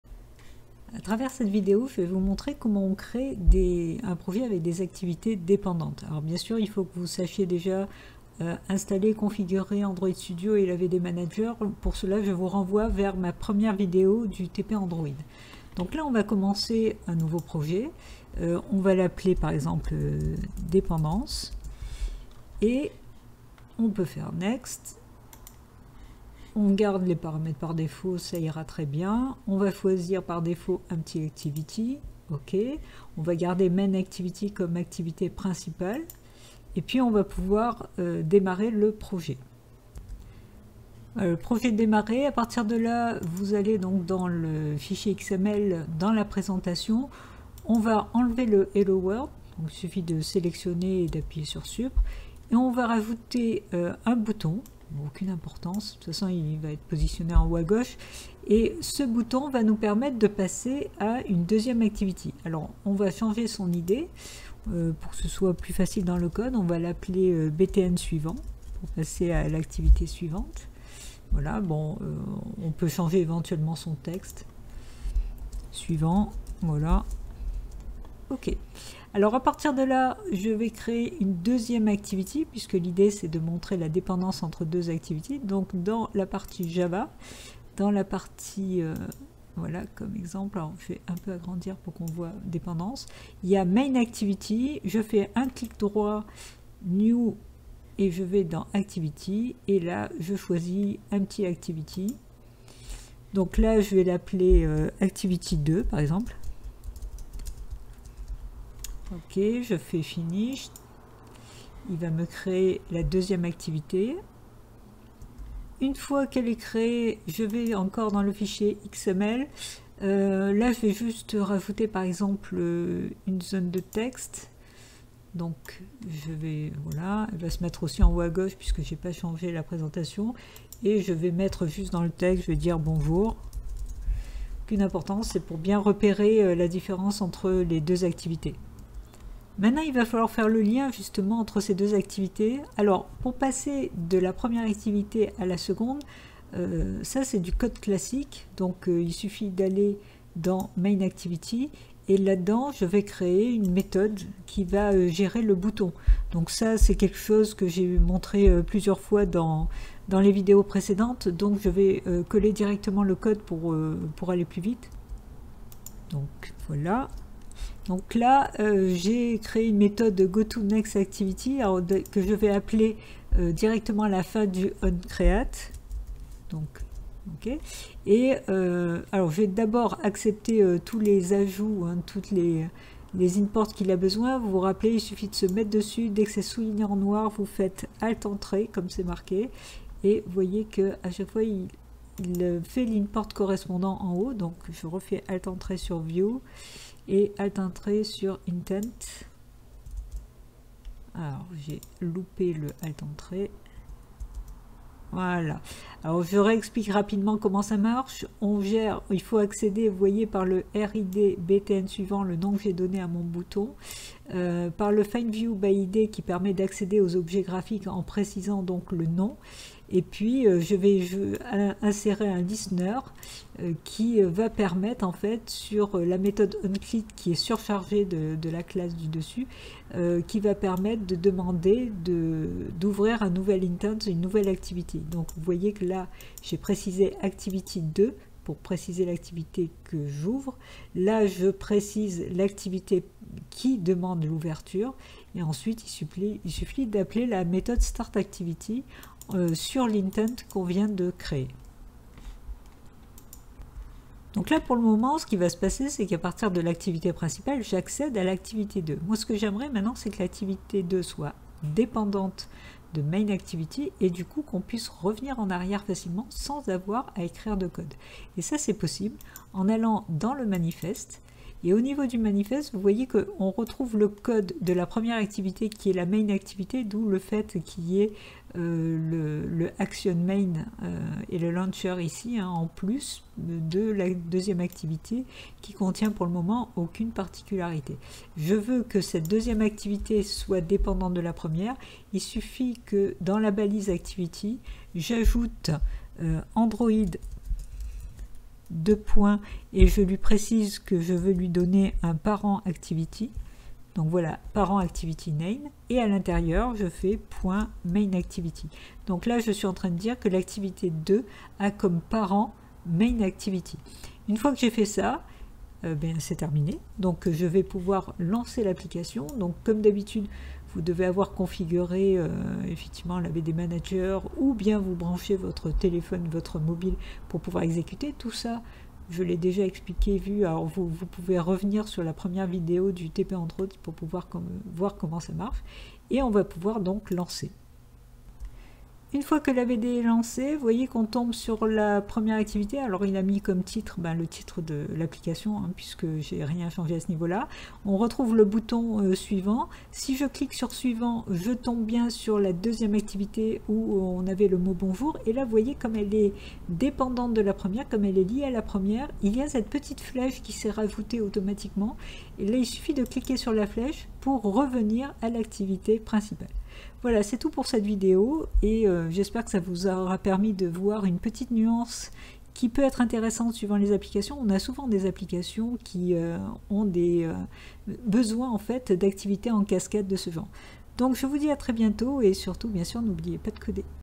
Android Studio (complément n°3) : Activity dépendante
Apprendre à créer des activity dépendantes (revenir à l'activity parent par la flèche du haut).
Prérequis : avoir installé et configuré Android Studio.